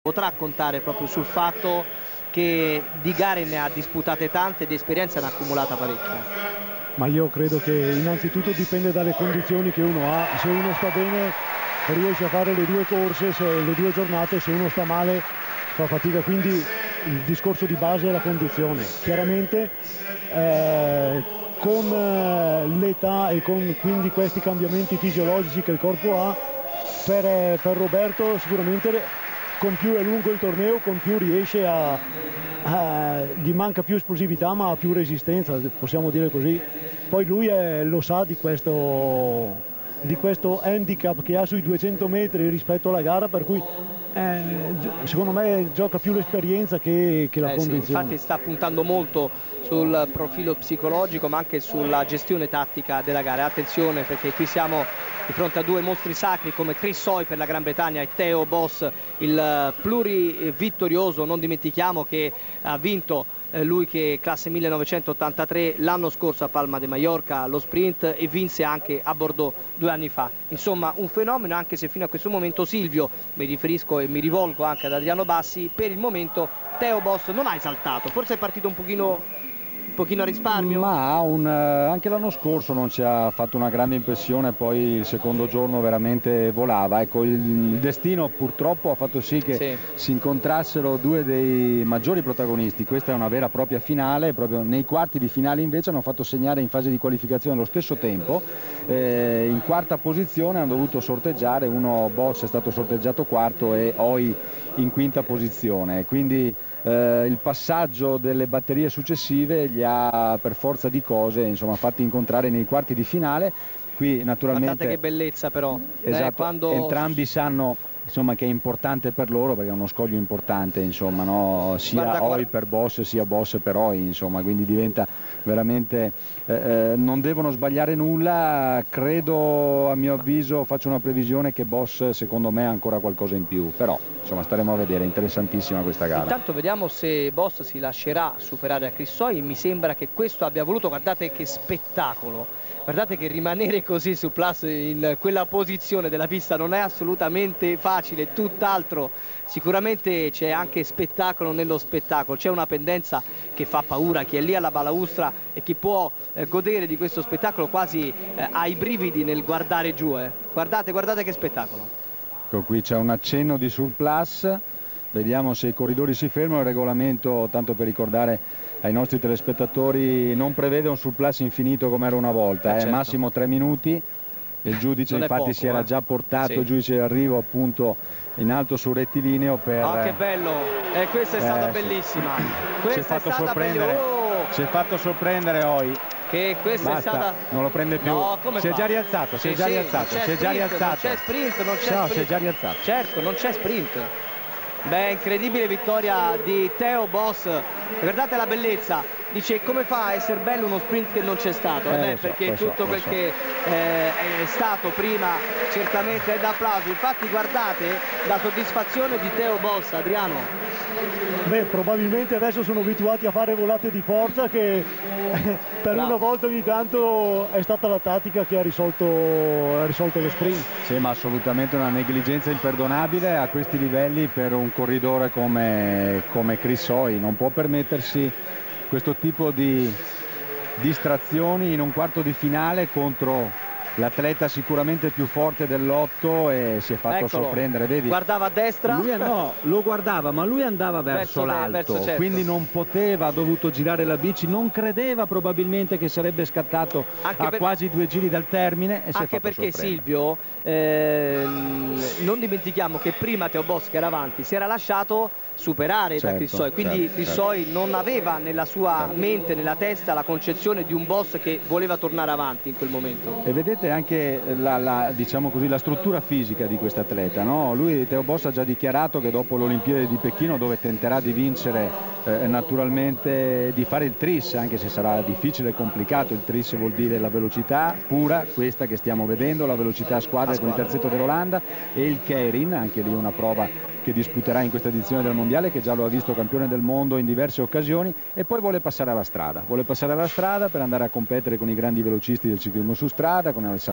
Chris Hoy vs Theo Bos - sprint world championchips '08 p1
Prima manche dei 1/4 di finale della velocità ai mondiali di Manchester 2008.
Le tre prove sono state di una spettacolarità unica.